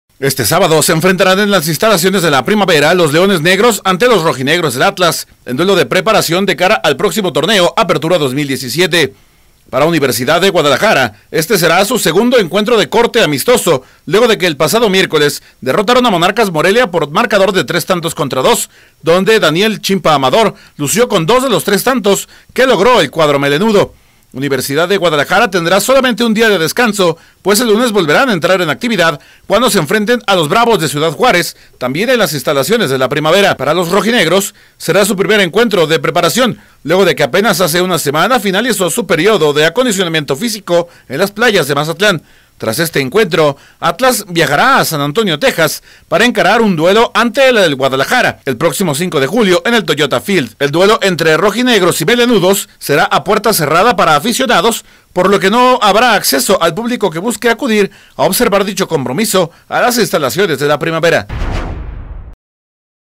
Leones Negros se reporta listo para enfrentar a los zorros del Atlas
Leones Negros se reporta listo para enfrentar a los zorros del

Atlas en partido amistoso en las instalaciones de la primavera.